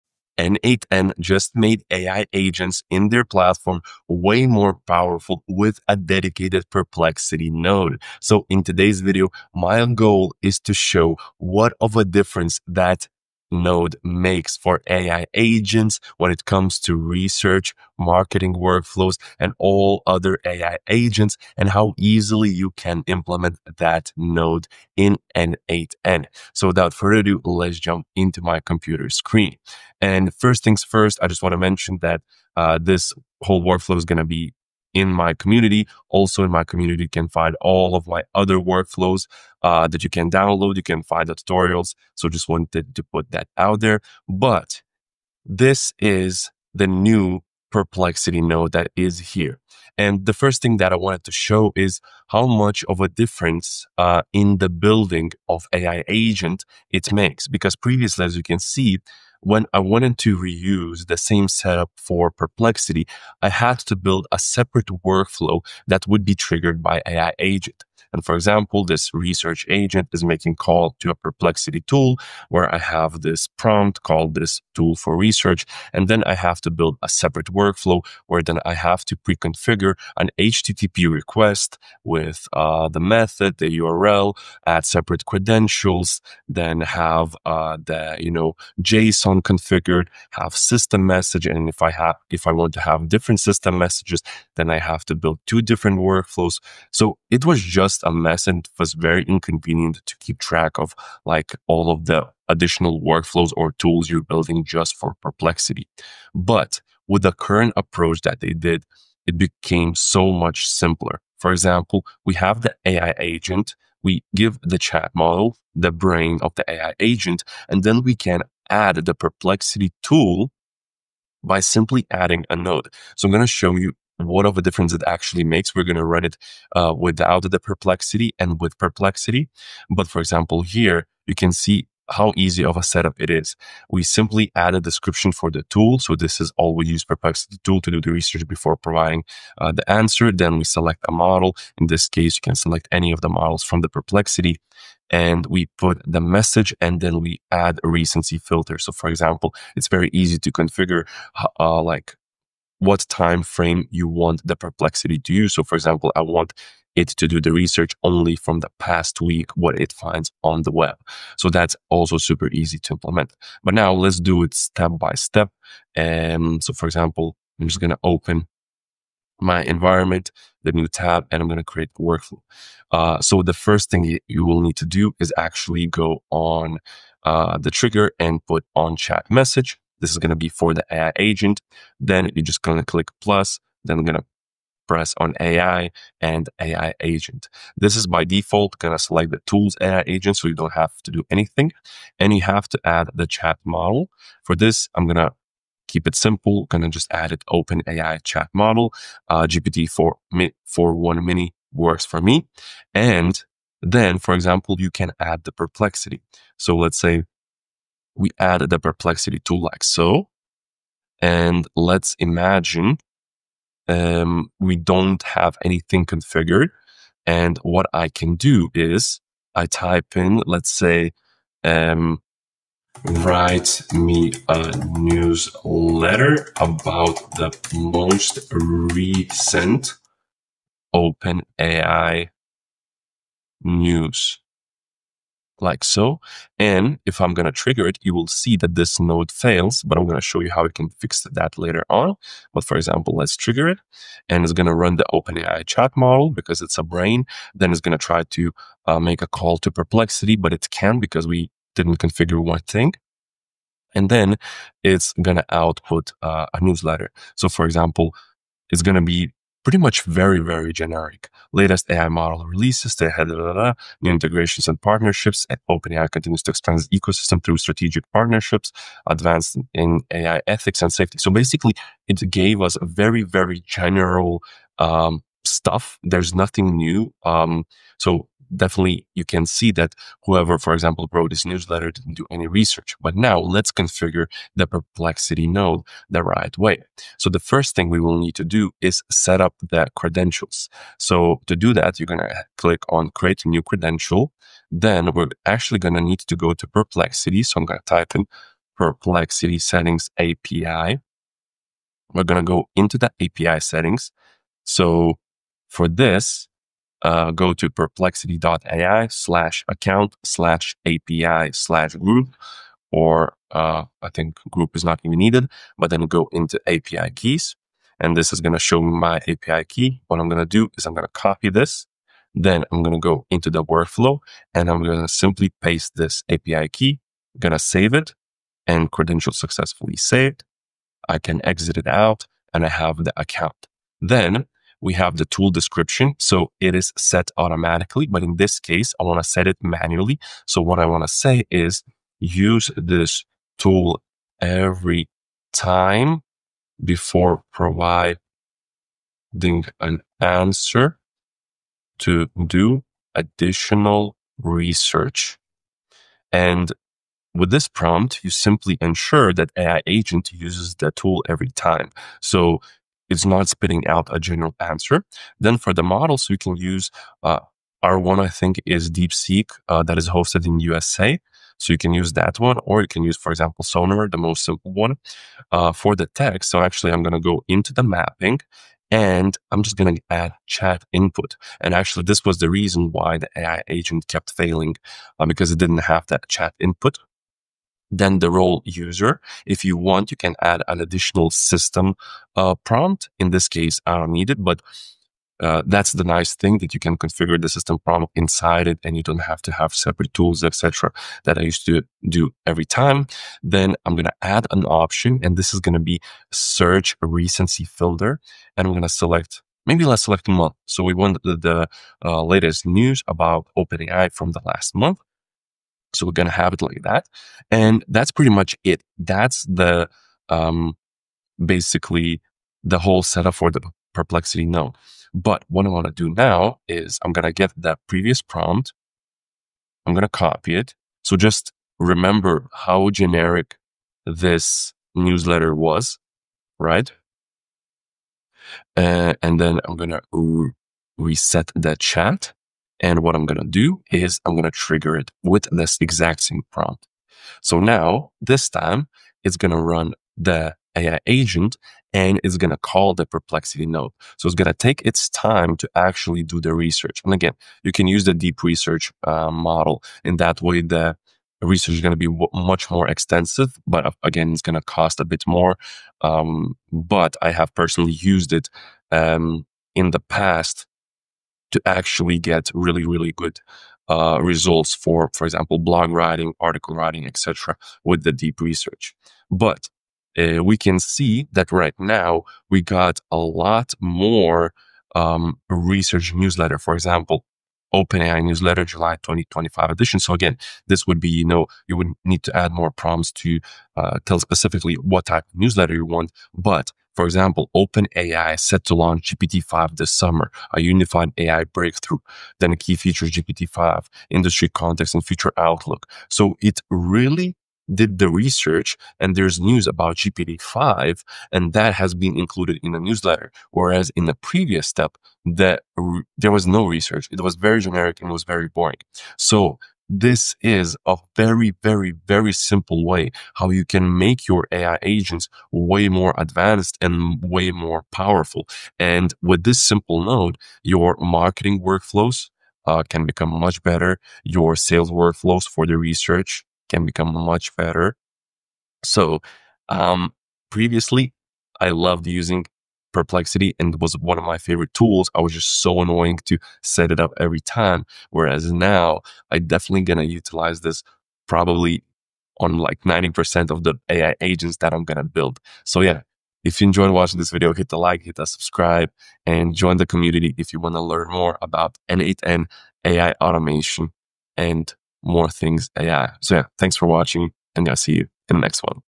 New Perplexity AI N8N Update is Insane! AI Agents Got Even Better
My tools & deals (some of these give me kickbacks—thank you!)

Timestamps:

00:00 Introduction
00:31 How you can get this and all other n8n templates from me
00:47 What has improved?
02:54 Setting up the AI Agent with Perplexity Tools
03:40 The AI Agent output comparison without Perplexity Tool
05:40 Configuring Perplexity Tool parameters and prompts in n8n
07:05 The improved quality of AI Agent with new Perplexity Tool